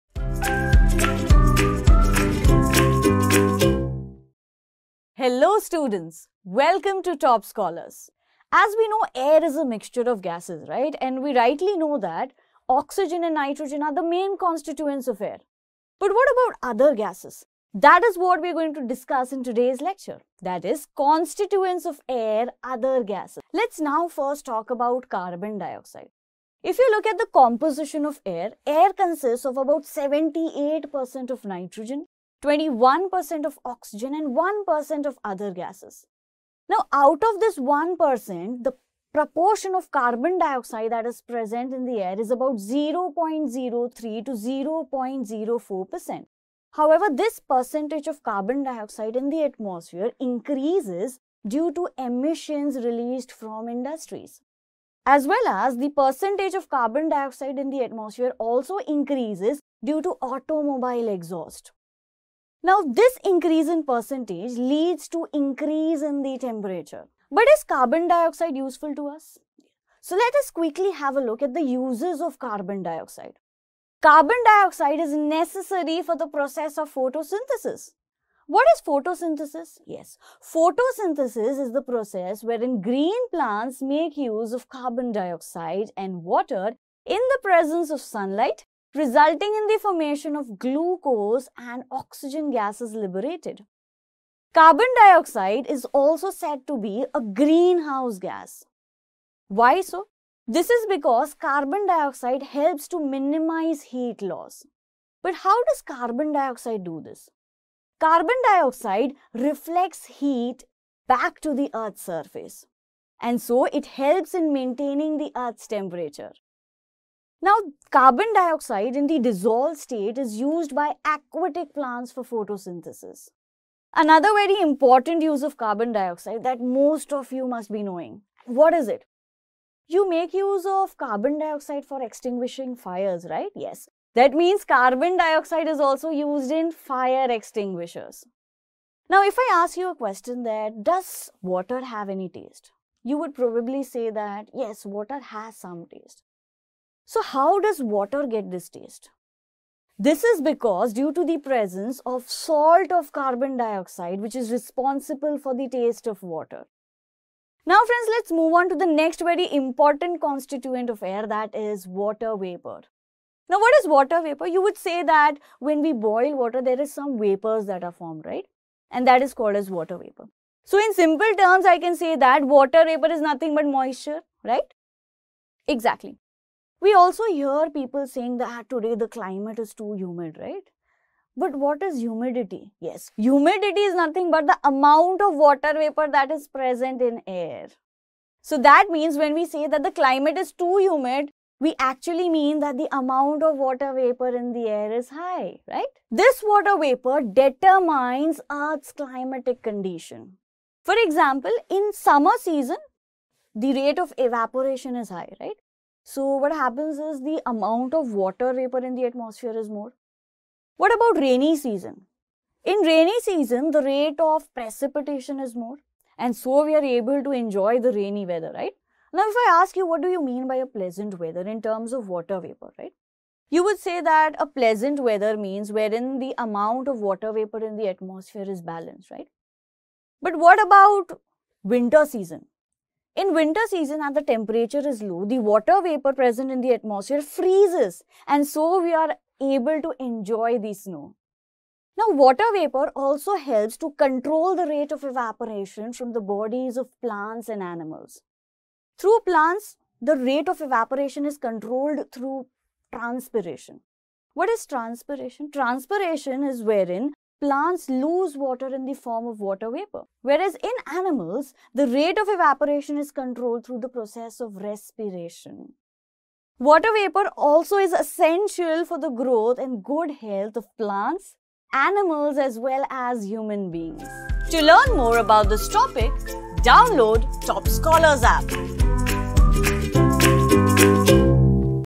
Constituents of Air: Other Gases | Chemistry
Air is a mixture of gases wherein nitrogen and oxygen are the main constituents. Air also contains carbon dioxide, water vapor, inert gases, and ozone in small proportions. This video tells us about the other gases present in air and also their uses.